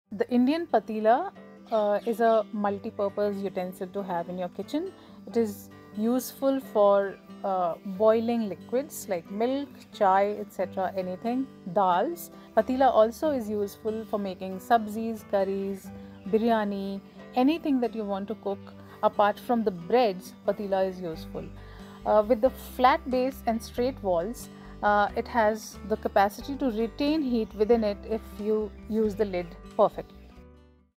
Multi Purpose Brass Patila | How can we use Brass Patila or Topia in kitchen | P-TAL Patila
The beauty of the Indian vessel of Patila lies in its ability of being able to be used in various kinds of cooking. From boiling liquids to cooking some tea, from tasty biryanis and rice to dals and even subzis; this is a one stop solution. The design of a patila is quite thoughtful as the flat base and higher walls help in retaining heat especially when covered with a lid properly. This definitely becomes a saviour in kitchen.